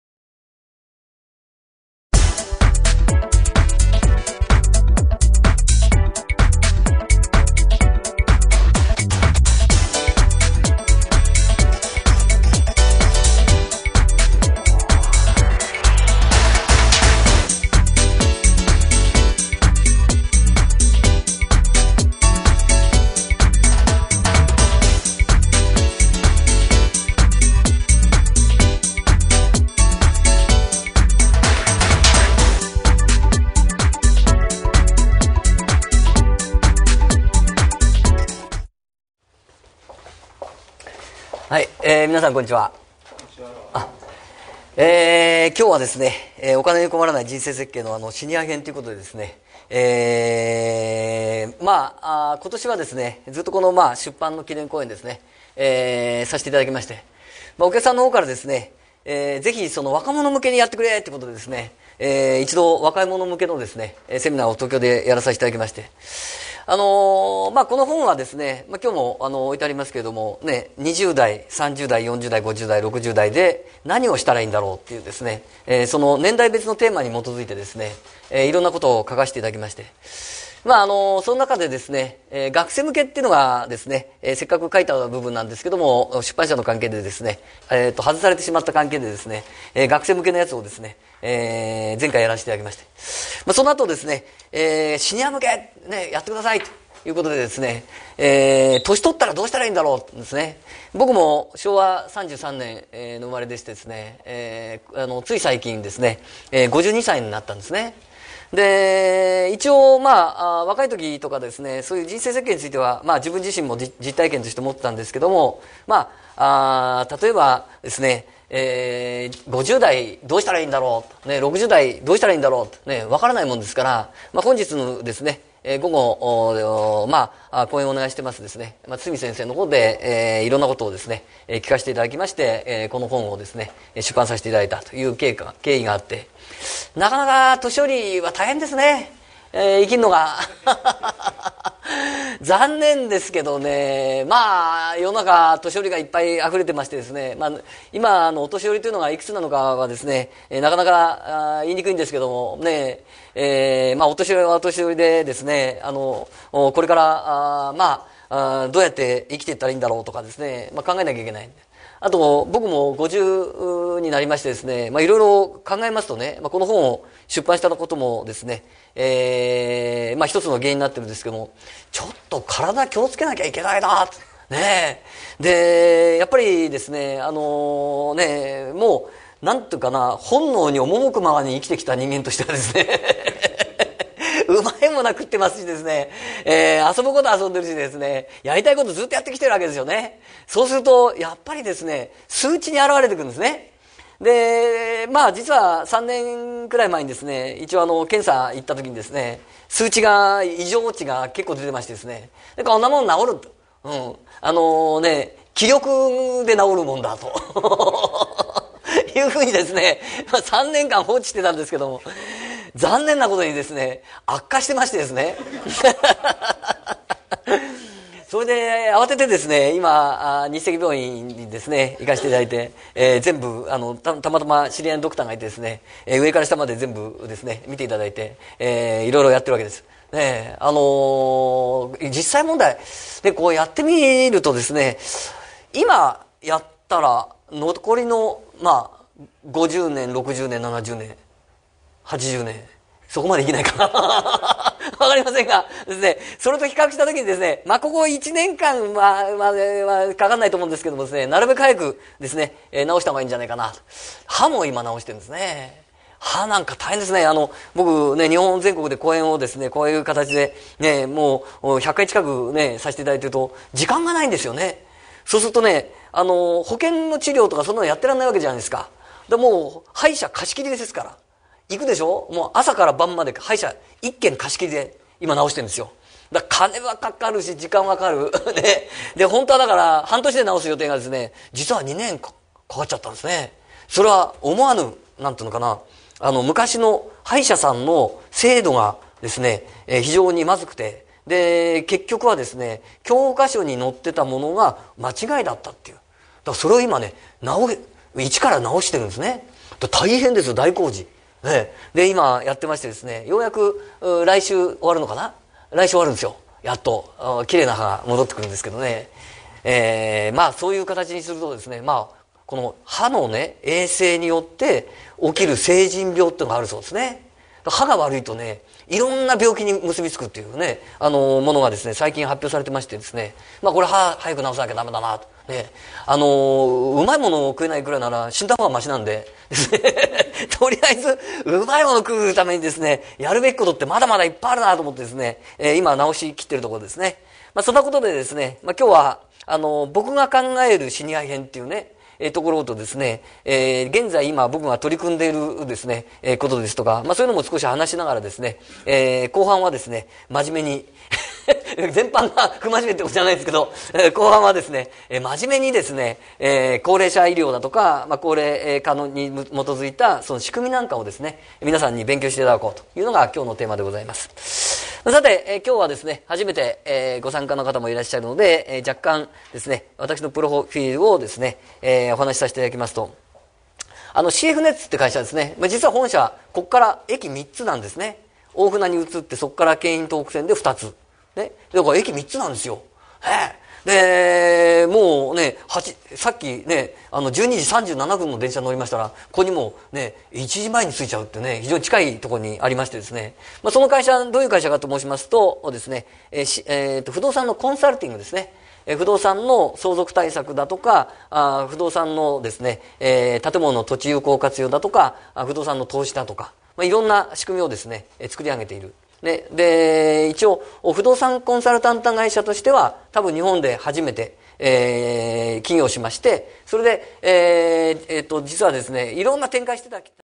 シニア向け お金に困らない人生設計
現在の日本は、他の先進国に比べ、高齢化が急激に進行しています。それに伴う高齢社会に対応すべき制度・政策を十分に練ることができないままとなっています。人間が歳をとることは当たり前のことで、たとえ年老いて病気をしても、身体が動かなくなっても、住み慣れた自分の家で暮らせればそれなりに満足できるかもしれませんが、実際はそう簡単なことではありません。

住宅事情の悪さ、福祉サービスの貧困、医療サービスと介護サービスの接続の悪さなど、歳をとればとるほど「我慢」が増える現実があります。それなら施設にでも入ろうかと考えますが、公的施設は不足していて年単位の待機を余儀なくされます。有料老人ホームにしたらよいのではと思っても、入居後に倒産したり、途中で経営者が変わって不便になったり、介護の手間がかかるようになったとたんに想定外の退去を要求され途方にくれる人も珍しくありません。

CFネッツグループ代表の著書「お金に困らない人生設計」の高齢者編の著者であり、現役ケアマネージャーのハートランドふじさわ代表つつみいくみ氏が、超高齢社会を生き抜く策を伝授。医療の問題、介護の問題、家族の問題など、実務体験を通して、問題解決のための心構えと考え方を本セミナーDVDに収録いたしました。

　株式会社シー･エフ･ネッツ　企画開発室
　〒234-0054 横浜市港南区港南台3丁目3番1号　港南台214ビル407号